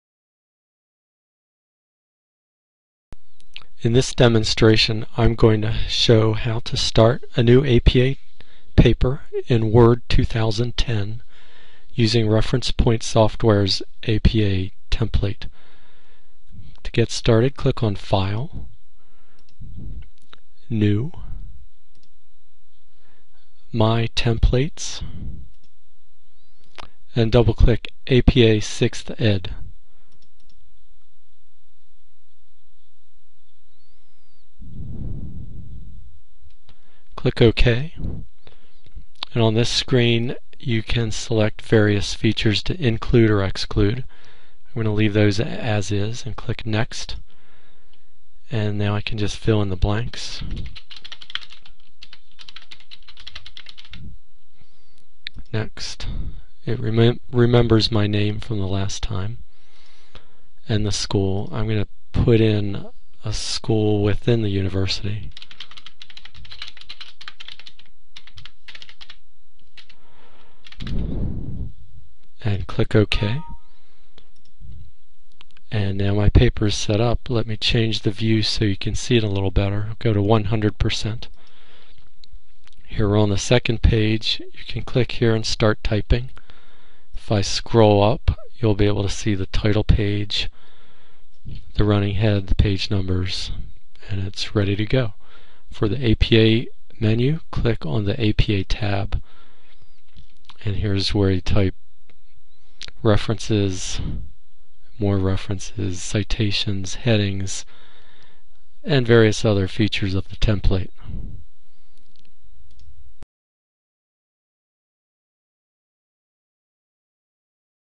APA Tutorial: Starting a new paper with Word 2010
Start a new APA paper using Reference Point Software and Word 2010